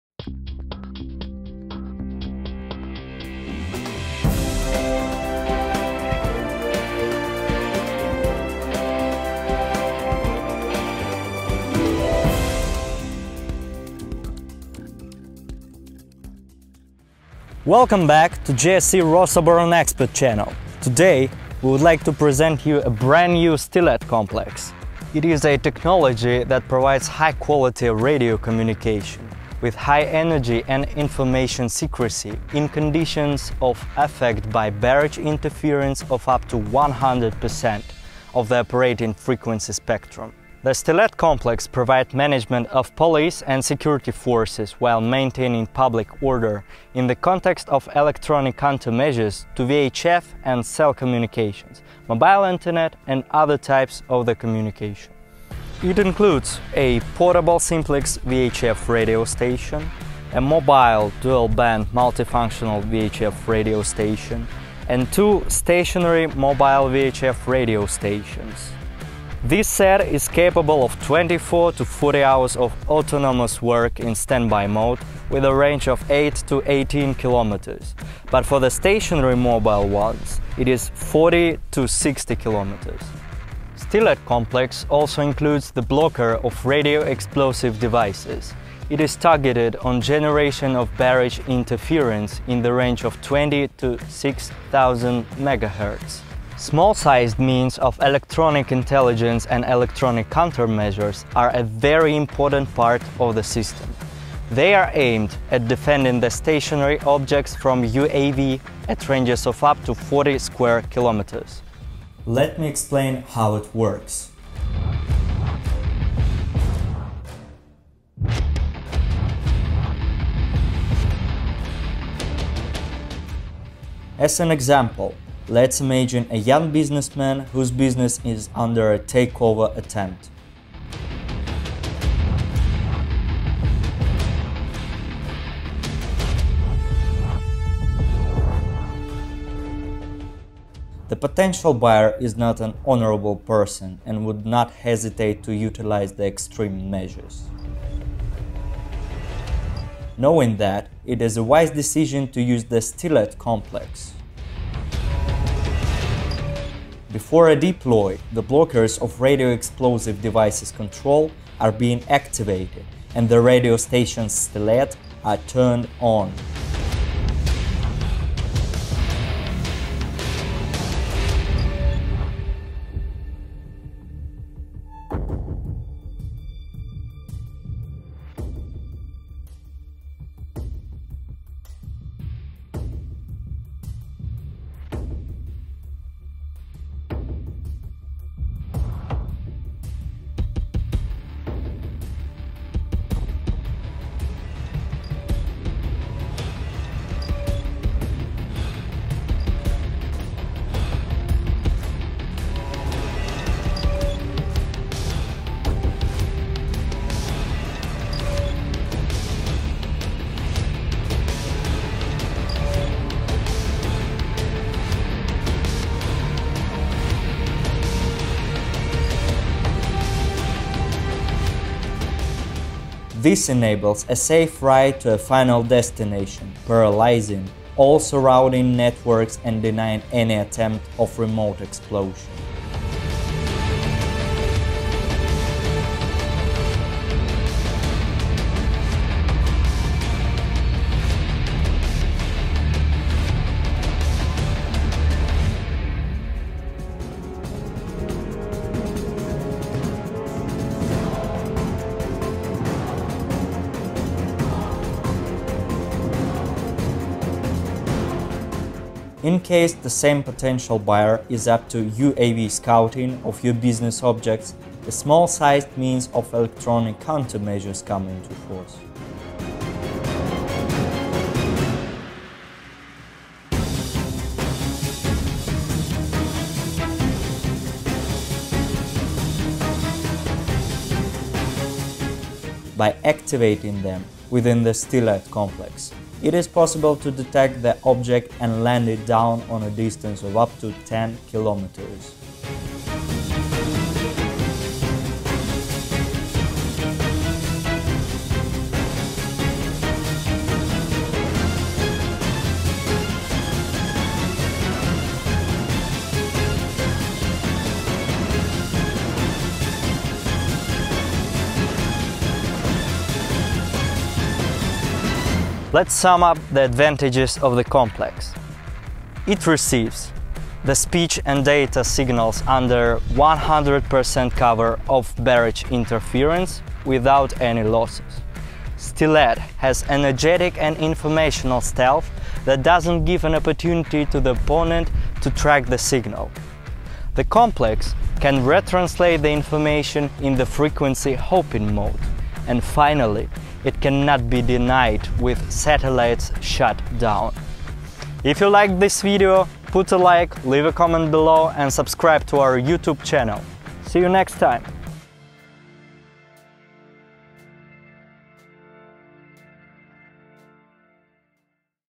Stilet radio communication system
Russian manufacturers have developed technology for receiving frequency hopping spread spectrum (FHSS) signals in barrage jamming environments, i.e. receiving signals when the entire operating frequency spectrum is jammed.
This technology is implemented in the STILET radio communication, navigation and electronic document management system.